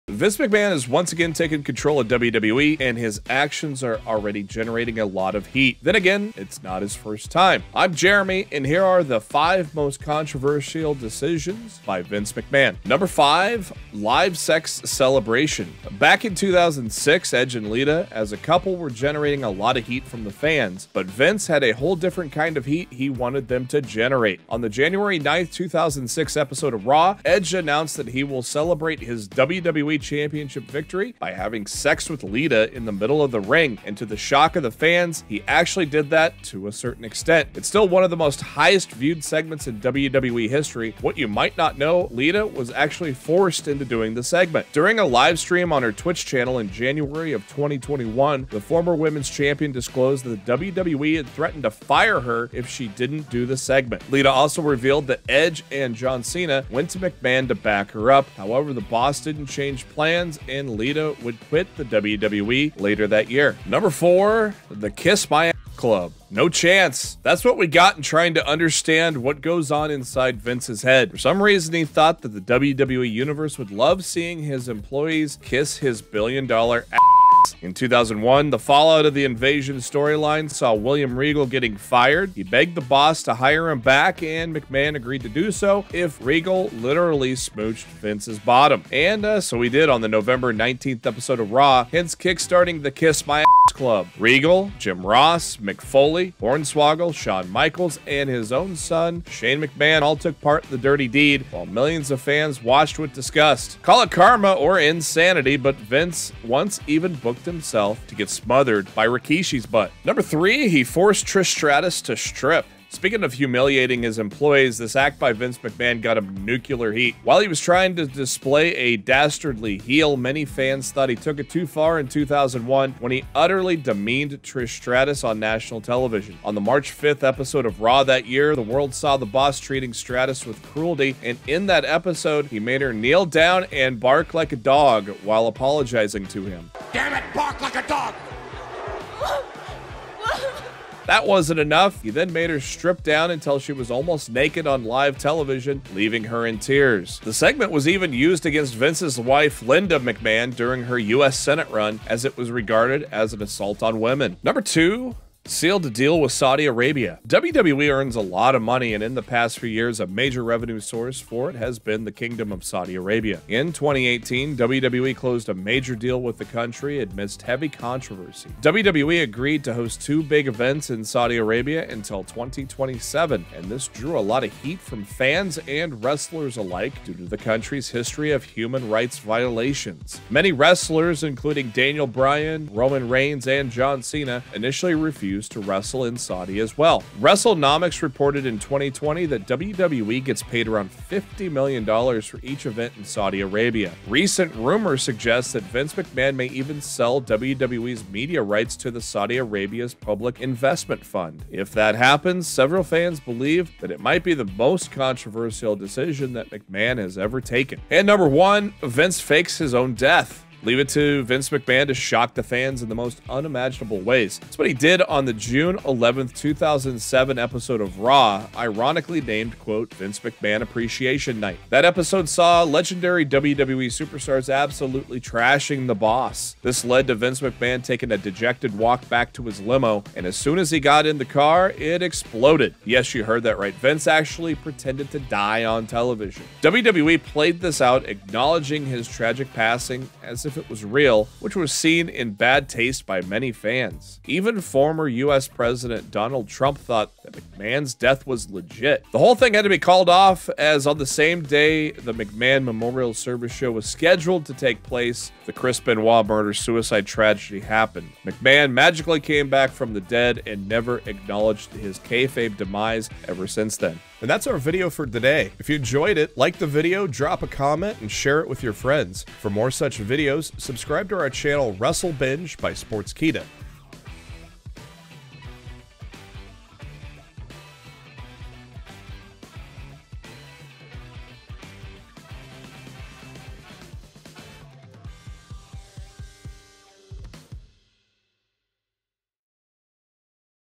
5 most controversial decisions by Vince McMahon | Edge, Lita, Trish Stratus & more
No chance! That's what we all got when we try to understand some decisions WWE's current Executive Chairman Vince McMahon has made in the past. Vince has often landed in controversy and has been a subject of a lot of heat from the fans for some of his choices. He's made his wrestlers do some dastardly things inside the ring which are hard to justify by reason. While ratings are truly king, some fans believe that Vince took it too far when he booked these segments or made the decisions.

In this video, we look at 5 controversial decisions by Vince McMahon.

Timestamps:-
00:00 - Introduction
00:13 - Live sex celebration
01:04 - Kiss my a** club
01:50 - Forced Trish Stratus to strip
02:35 - Sealed a deal with Saudi Arabia
03:28 - Faked his own death